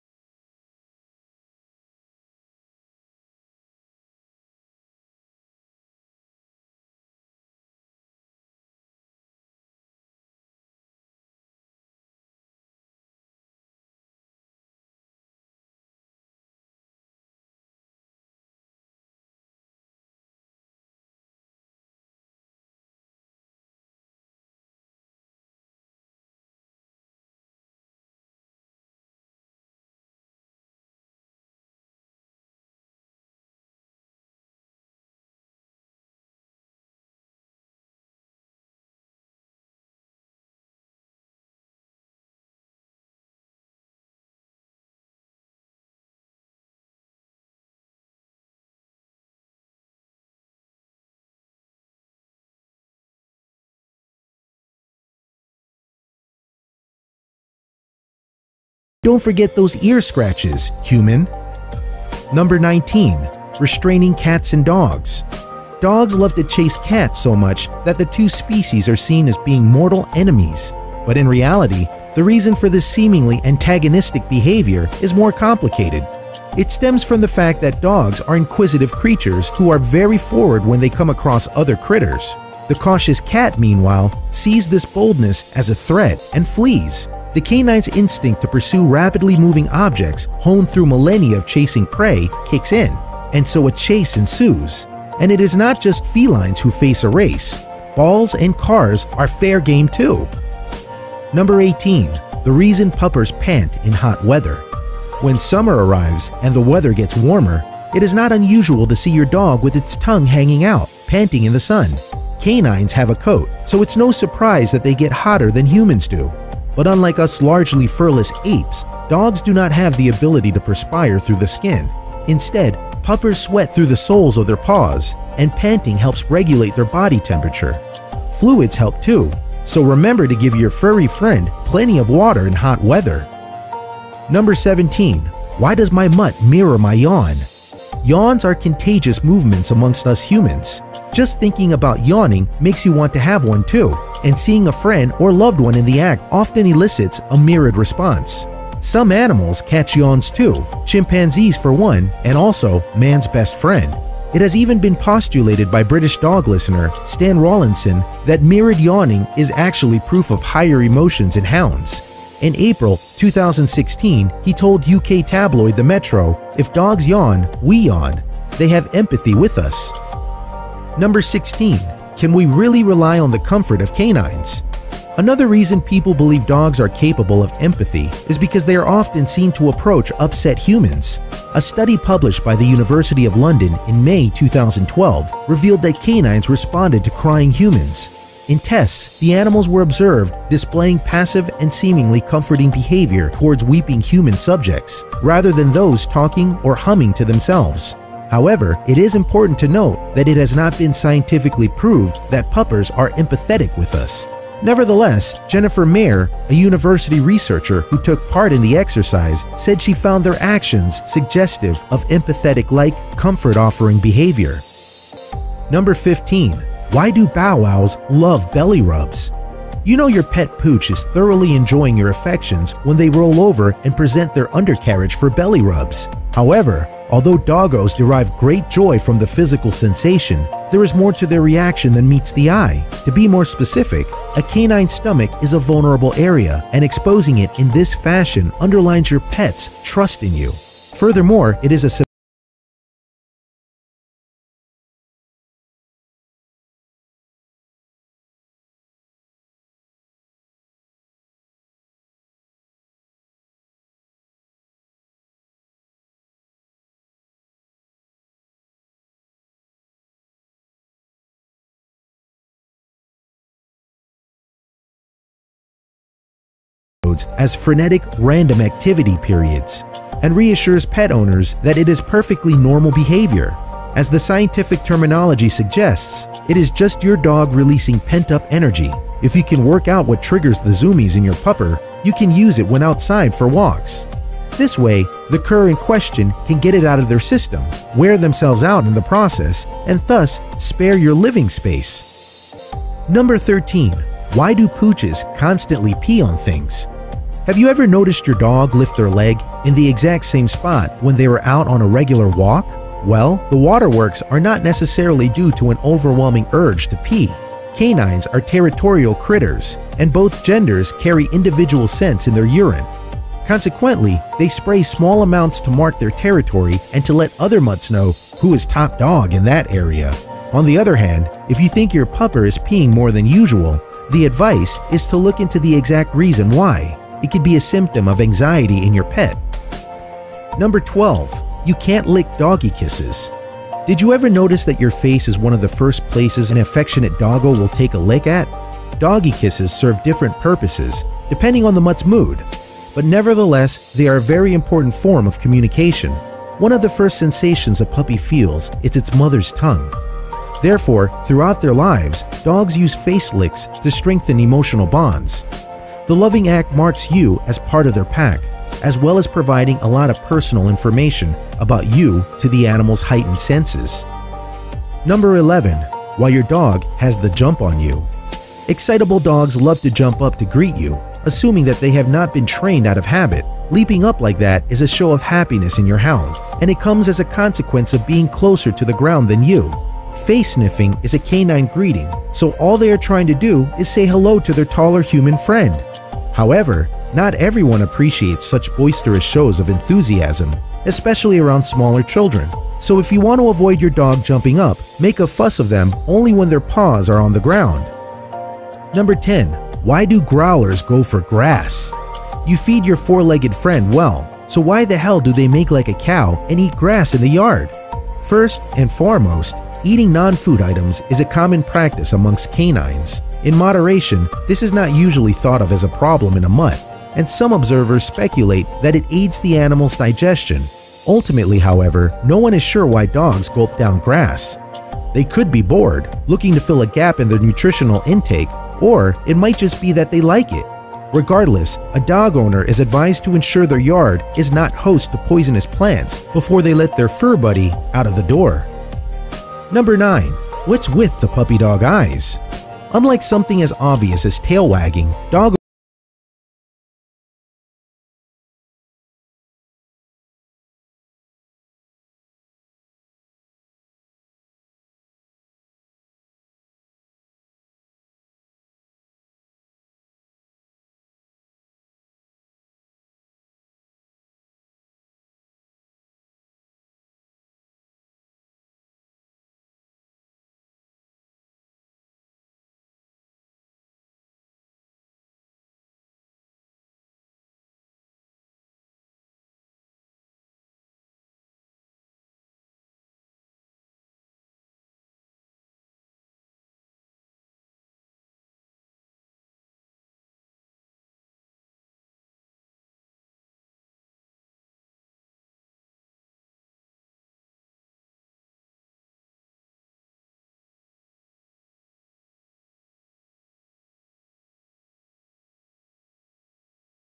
This Is Why Dogs Follow You Into The Bathroom. I Never Knew This!
We welcome dogs into our homes, our lives and our families to share some of our most personal moments. But what do we really know about the mutt in our midst? What’s going on behind those adorable puppy-dog eyes, and why do they insist on watching you poop? Don’t be dogged by insecurity at being left in the dark; here is an enlightening list of 20 bow-wow behaviors laid bare…

20. Why do pooches lean against you and give you paws?
You’re watching TV, and you suddenly feel the extra weight of your dog pushing against your leg. Perhaps the pupper even pats at you with a proffered paw – what’s that all about? Well, it means that your pet wants your love. Such behavior is a sign of doggy devotion, and the obvious answer is they just want to be close to you. It is also a subtle request for more affection – so ignore the screen and don’t forget those ear scratches, human!